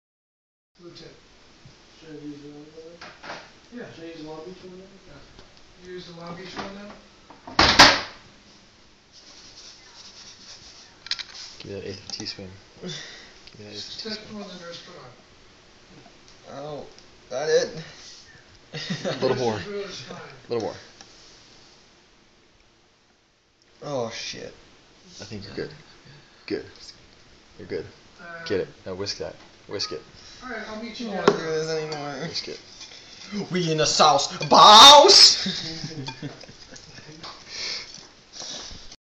Dusty sous chef
First helps with the Thanksgiving dessert, I think.